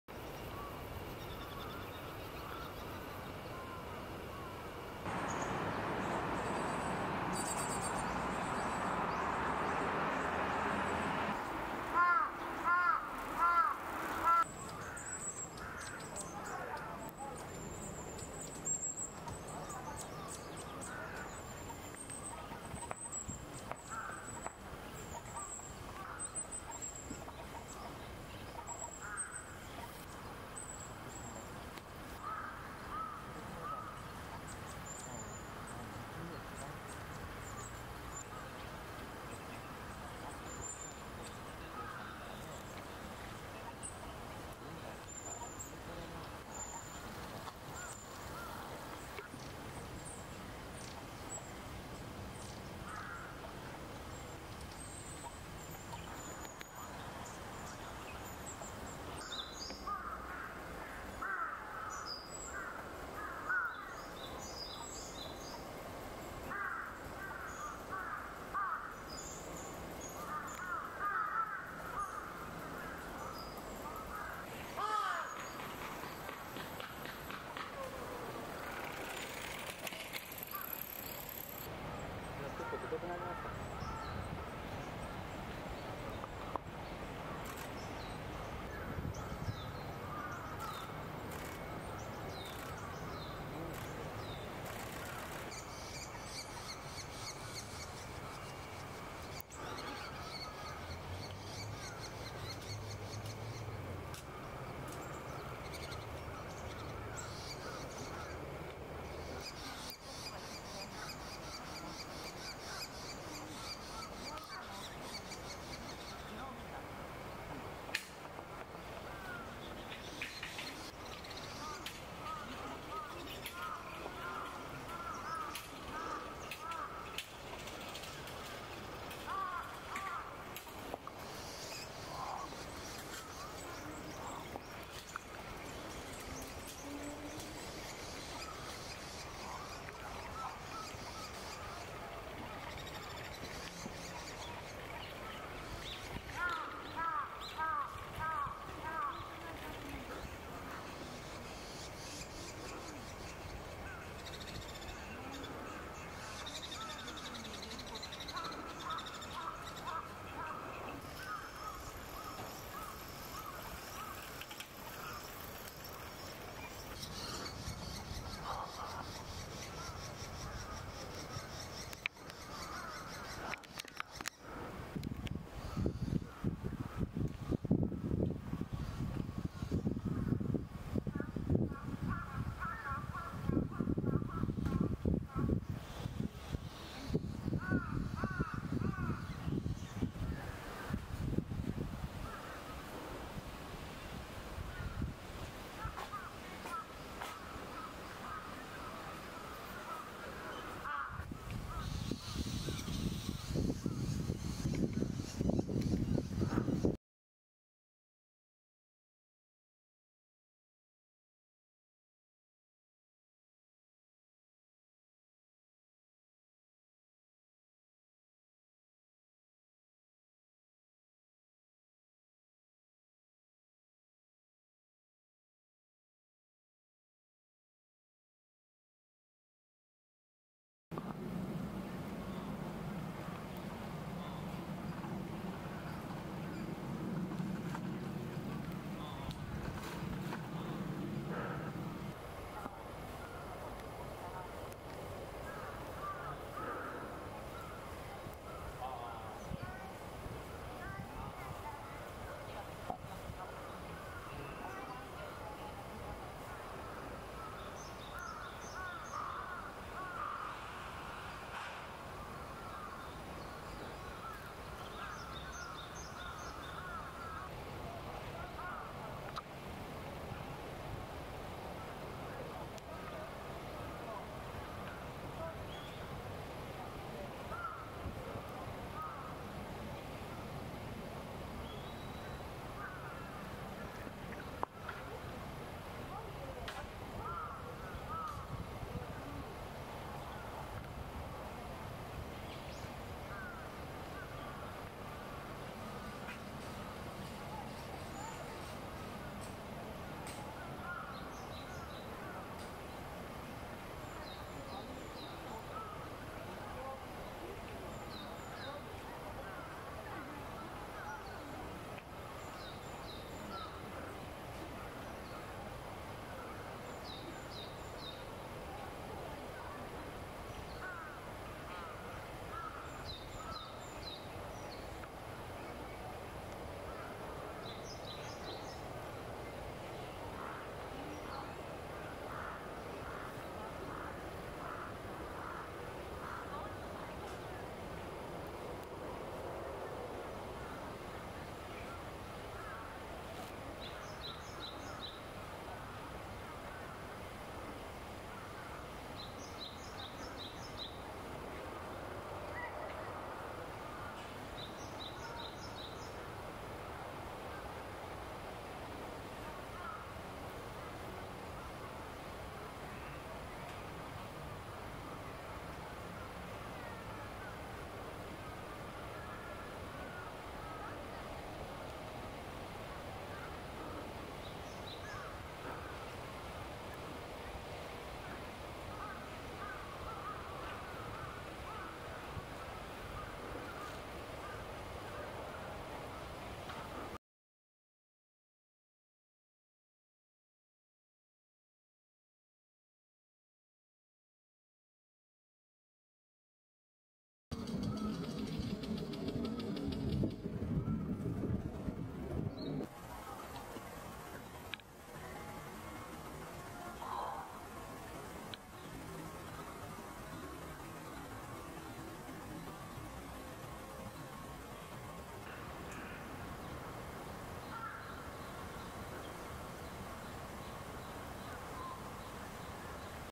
野鳥撮影 エナガ コゲラ モズの親子 水元公園
2021年5月8日 水元公園#モズの親子 エナガ コゲラの#モズの幼鳥 野鳥撮影